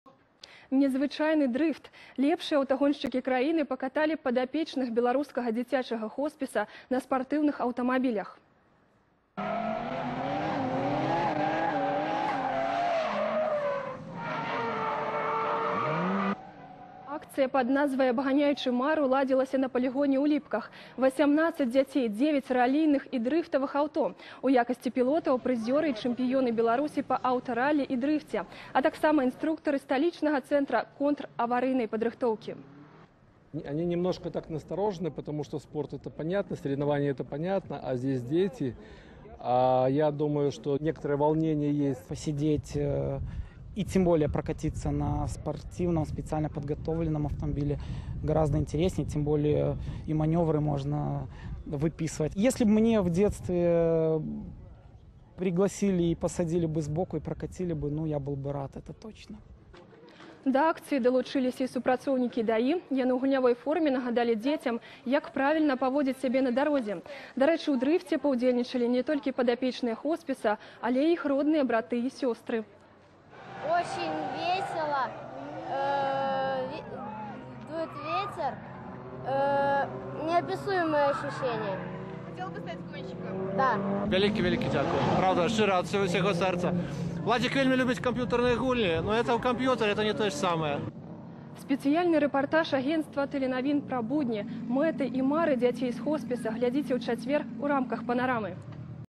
Незвычайны дрыфт!
Лепшыя аўтагоншчыкі Беларусi пакаталі падапечных дзіцячага хоспіса на спартыўных аўтамабілях.